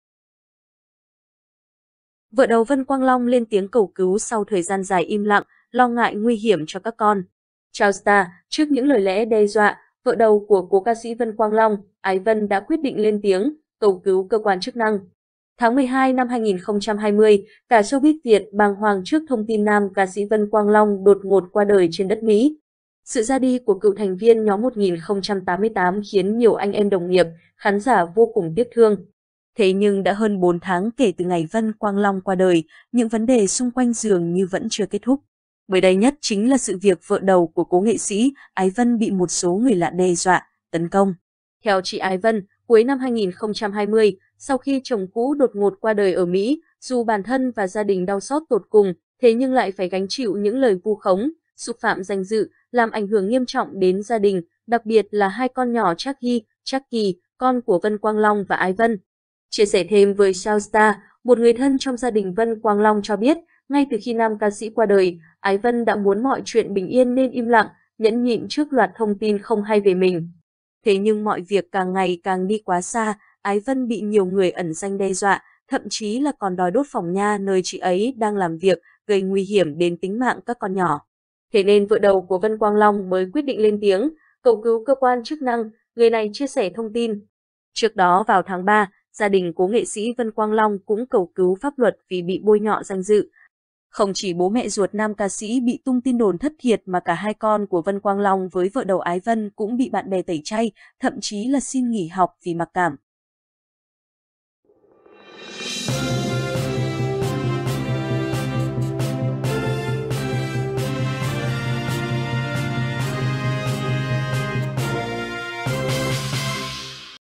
Ái Vân Vợ đầu Vân Quang Long lên tiếng cầu cứu sau thời gian dài im lặng
Tháng 12/2020, cả showbiz Việt bàng hoàng trước thông tin nam ca sĩ Vân Quang Long đột ngột qua đời trên đất Mỹ. Sự ra đi của cựu thành viên nhóm 1088 khiến nhiều anh em đồng nghiệp, khán giả vô cùng tiếc thương.

Vân Quang Long bất ngờ qua đời tại Mỹ vào tháng 12/2020

Thế nhưng đã hơn 4 tháng kể từ ngày Vân Quang Long qua đời, những vấn đề xung quanh dường như vẫn chưa kết thúc. Mới đây nhất chính là sự việc vợ đầu của cố nghệ sĩ - Ái Vân bị một số người lạ đe dọa, tấn công.

Theo chị Ái Vân, cuối năm 2020, sau khi chồng cũ đột ngột qua đời ở Mỹ, dù bản thân và gia đình đau xót tột cùng, thế nhưng lại phải gánh chịu những lời vu khống, xúc phạm danh dự, làm ảnh hưởng nghiêm trọng đến gia đình, đặc biệt là hai con nhỏ Trác Hy - Trác Kỳ (con của Vân Quang Long và Ái Vân).

Chia sẻ thêm với SAOstar, một người thân trong gia đình Vân Quang Long cho biết, ngay từ khi nam ca sĩ qua đời, Ái Vân đã muốn mọi chuyện bình yên nên im lặng, nhẫn nhịn trước loạt thông tin không hay về mình.

Ái Vân cầu cứu cơ quan chức năng sau khi nhận nhiều lời đe dọa

"Thế nhưng mọi việc càng ngày càng đi quá xa, Ái Vân bị nhiều người ẩn danh đe dọa, thậm chí là còn đòi đốt phòng nha nơi chị ấy đang làm việc, gây nguy hiểm đến tính mạng các con nhỏ. Thế nên vợ đầu của Vân Quang Long mới quyết định lên tiếng, cầu cứu cơ quan chức năng", người này chia sẻ thông tin.

Trước đó vào tháng 3, gia đình cố nghệ sĩ Vân Quang Long cũng cầu cứu pháp luật vì bị bôi nhọ danh dự. Không chỉ bố mẹ ruột nam ca sĩ bị tung tin đồn thất thiệt mà cả 2 con của Vân Quang Long với vợ đầu Ái Vân cũng bị bạn bè tẩy chay, thậm chí là xin nghỉ học vì mặc cảm.